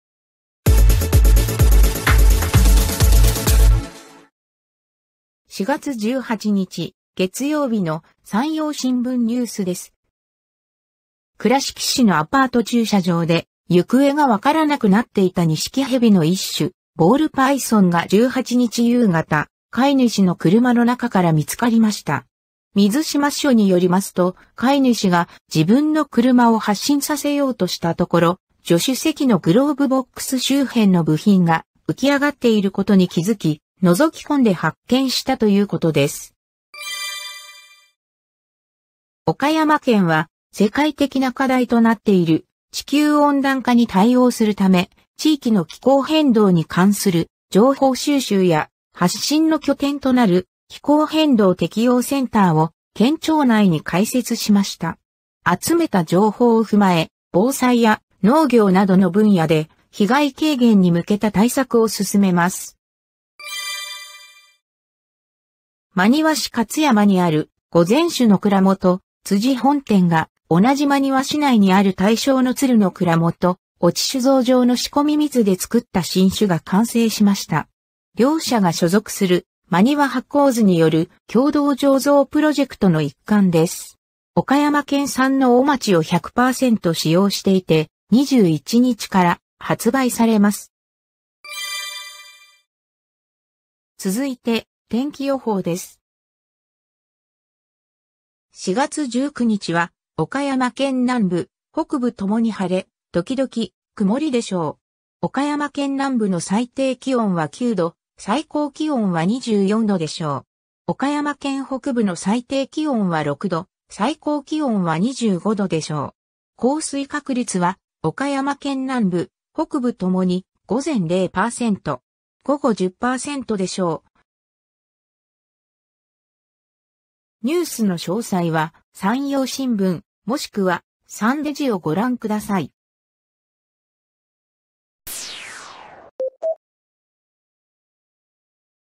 【４月１８日】不明のニシキヘビ見つかる ＡＩアナＮＥＷＳ
1,不明のニシキヘビ見つかる
2,気候変動適応センターを開設
3,辻本店、中硬水で新酒